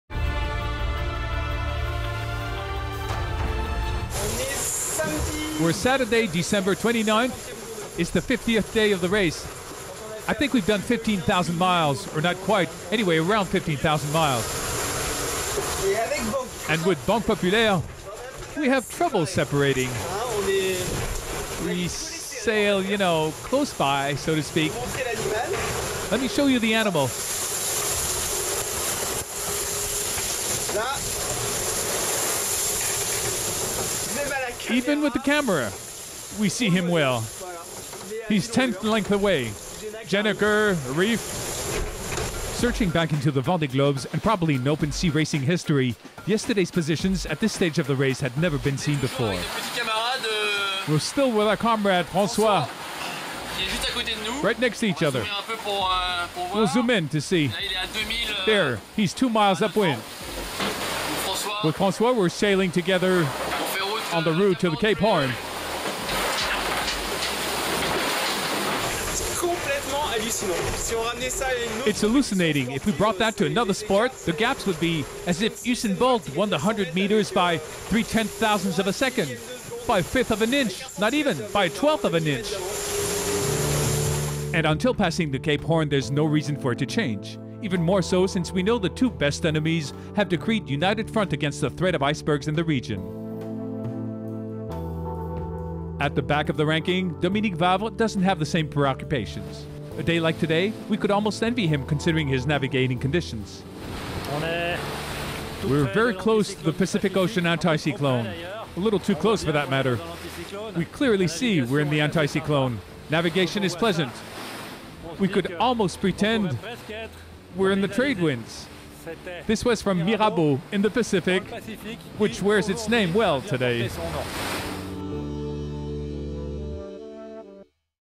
Vendée Globe - Day 51 highlights - the leaders sail together after 51 days at sea
Here is the highlights video of the 51th day of the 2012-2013 Vendée Globe - Sunday, December 30, 2012. the leaders François Gabart (Macif) and Armel Le Cleach (Banque Populaire) sail together after 51 days at sea.

The Vendée Globe 2012 is a singlehanded race around the world witthout assistance.The skippers round the Cape of Good Hope, the Cape leeuwin and Cape Horn. The start was given in the Sables d'Olonne, November 10th 2012.